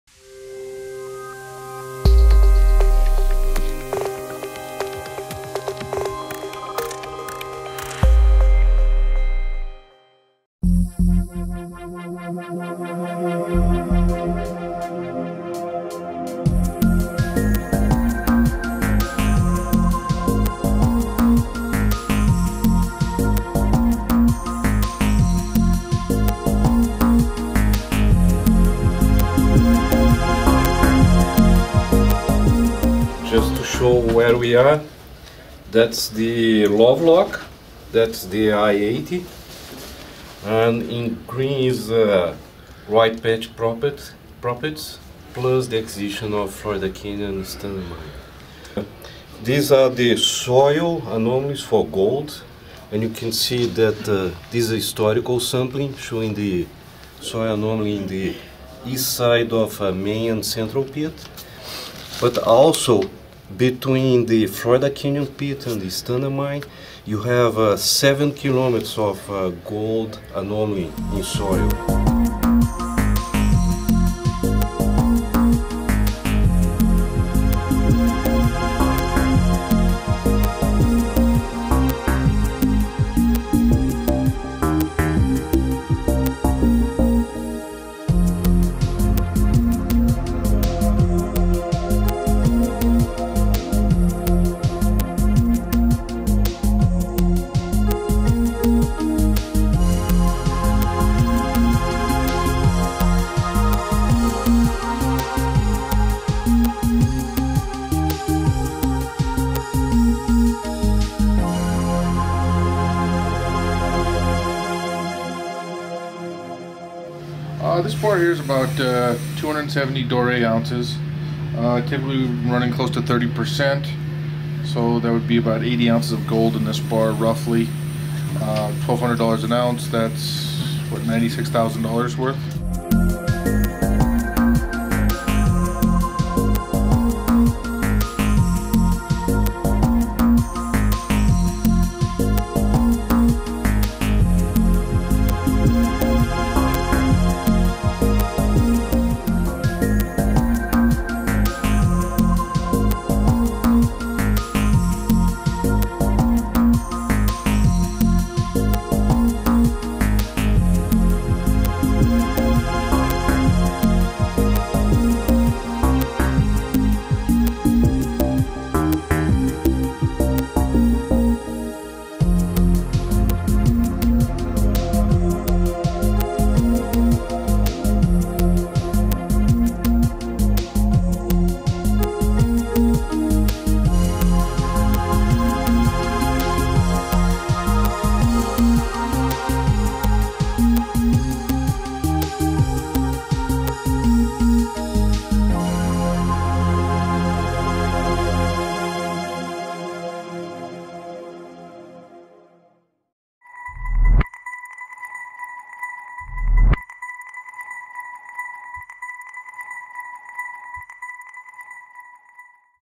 Rye Patch Gold Sitevisit, February 2017
Rye Patch Gold Sitevisit, February 2017. Impressions from the mining operations.